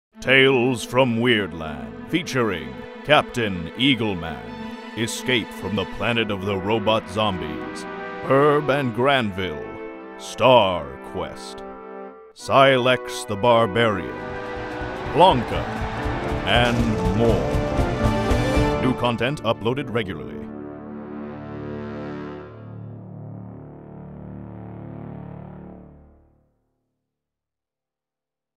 Tales from Weirdland (Channel Trailer)
Channel trailer for Tales from Weirdland The channel features animated videos by Jeronimus Dekker.

Background information (sketches, history, animation sequences, et cetera) on the videos

Though I've always liked to draw off and on, I've taken up animation only fairly recently. The channel contains the first fruits of my labor. I'm a one-man studio basically, self-taught, doing the writing, designing, animation, and music myself. I work in the old-fashioned way, pencil on paper, then scan my drawings and color them digitally. The software I use is all quite basic, because I do most of the work manually (and because I’m not bright enough to understand most software).

The channel contains absurdist stuff ("Lonely Zernak", "Plonka"), and more ambitious videos ("Captain Eagle Man", "Escape from the Planet of the Robot Zombies”).

All the videos are personal. I don't make them to wow an audience or to become a really famous YouTube Star. I make what I want to watch myself. And I'll have you know that my videos are thoroughly enjoyed by the 9 people who watch them.

Let's see... Linked to Instagram, Tumblr blog, told a bit about myself, antagonized the viewers... I think that's it. Oh, I thought I saw Rutger Hauer at the supermarket yesterday but having thought some more about it now, I don't think it was him.

You're still reading this.

You know, in the future there'll be world-famous celebrities that almost nobody has heard of. Cyan and magenta are the new orange and teal. "Art is never finished, only abandoned."